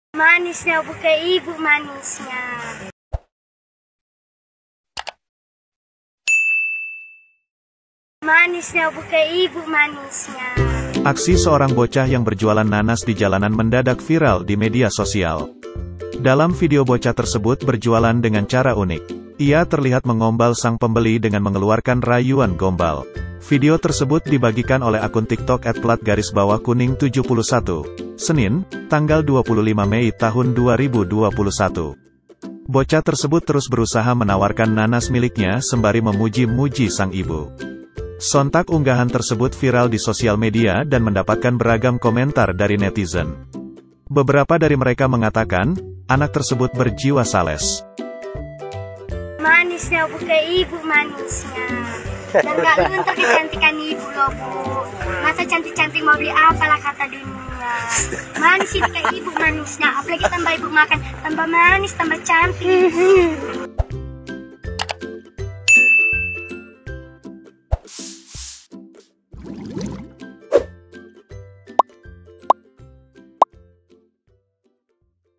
Rayuan Gombal, Bocah Penjual Nanas Bikin Pembeli Klepek-klepek
Dalam video bocah tersebut berjualan dengan cara unik. Ia terlihat mengombal sang pembeli dengan mengeluarkan rayuan gombal.

Video tersebut dibagikan oleh akun TikTok @plat_kuning71, Senin (25/5/2021).

Video terkini dan terbaru..